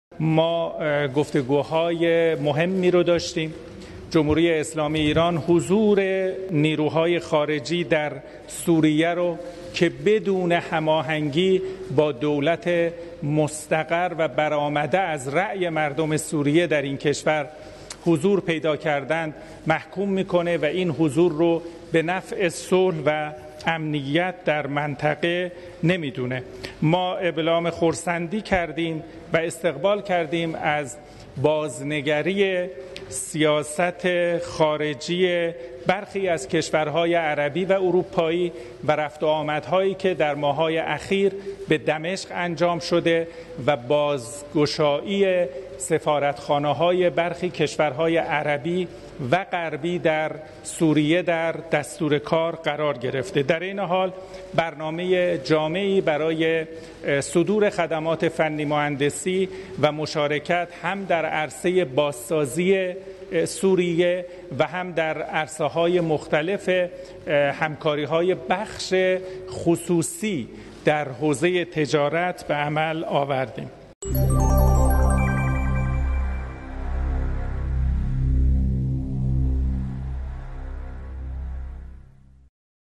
Iran slams foreigners presence in Syria
The foreign minister, in a meeting with his Syrian counterpart, said that Iran condemned foreign forces' presence in Syria without coordination with Damascus.